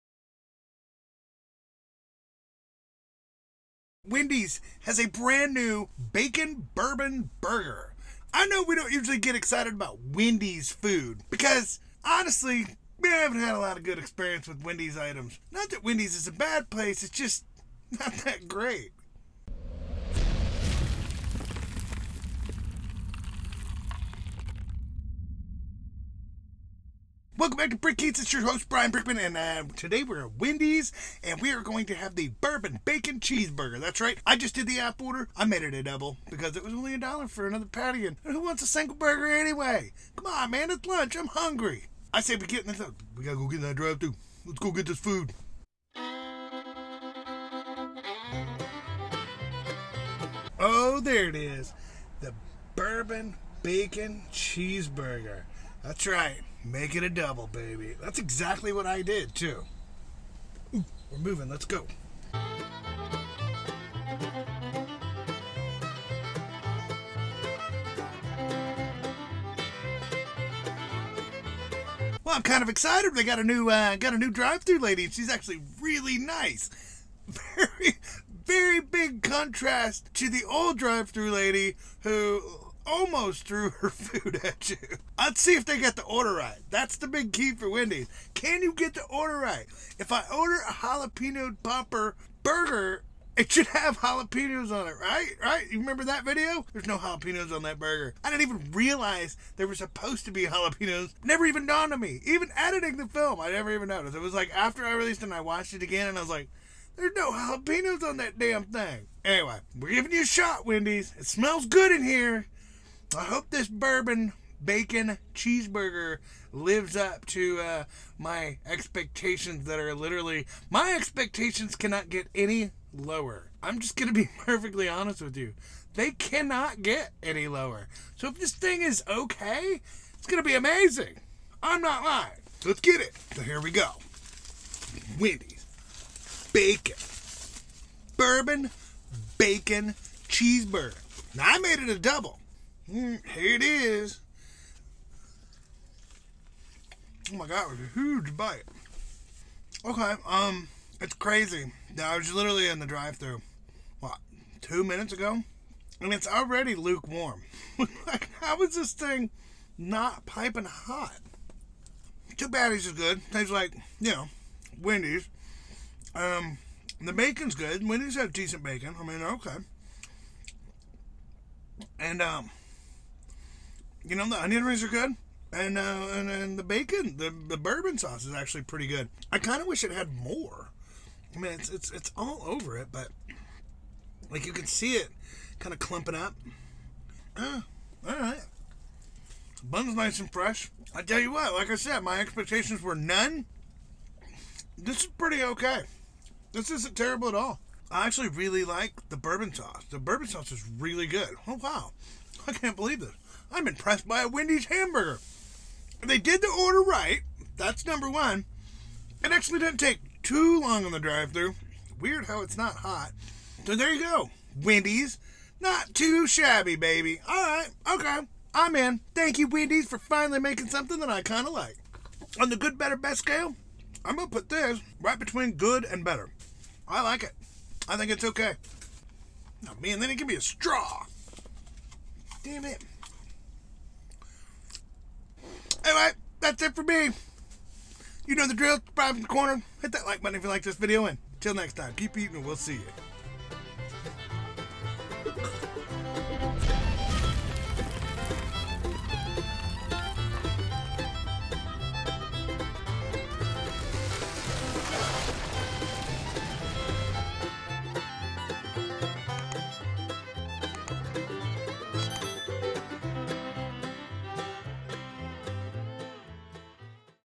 Brickeats Wendy's New Bourbon Bacon Cheeseburger
Wendys has an all new burger. again. Is this one going to be good??? Lets eat the Bourbon Bacon Cheeseburger and find out!!!